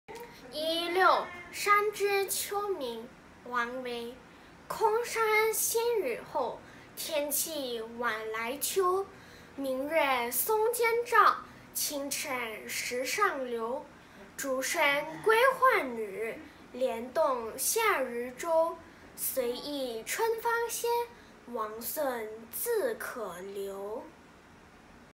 《唐诗三百首》320之116《山居秋暝》王维
空山新雨后，天气晚来秋。
明月松间照，清泉石上流。
竹喧归浣女，莲动下渔舟。
随意春芳歇，王孙自可留。